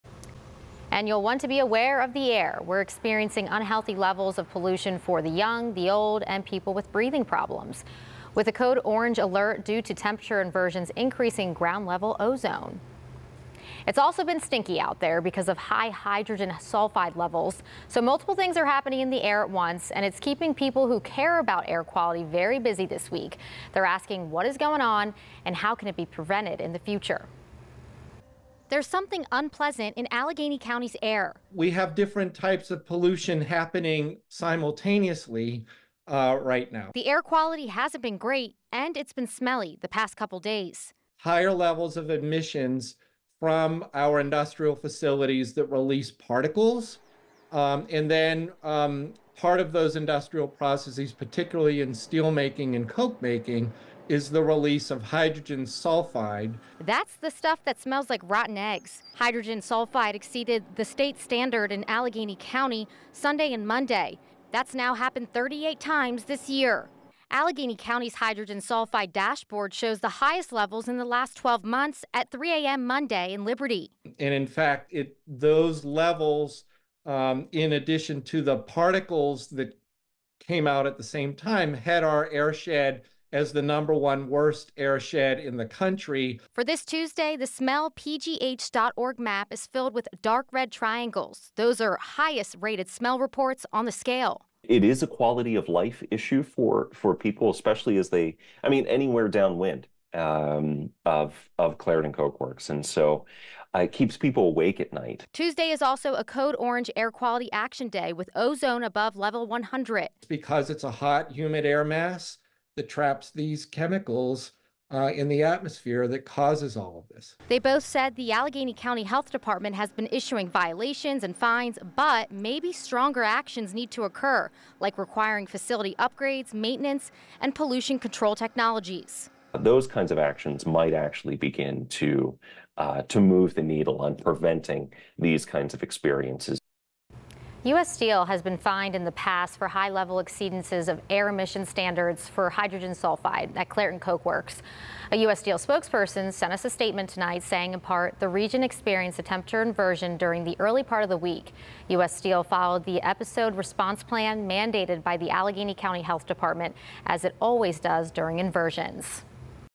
Poor air quality and bad smells consume parts of Southwestern Pennsylvania
Parts of Western Pennsylvania are experiencing poor air quality due to temperature inversions increasing ground-level ozone, and it's been stinky outside because of high hydrogen sulfide levels.